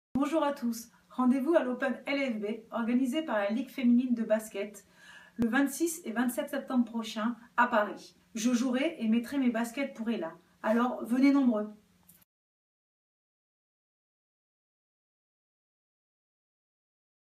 Open LFB 2015 - Paoline Salagnac vous donne rendez-vous !
La joueuse de Bourges et marraine d'ELA vous donne rendez-vous le samedi 26 et le dimanche 27 septembre au Stade Pierre-de-Coubertin de Paris pour l'‪#‎OpenLFB‬ !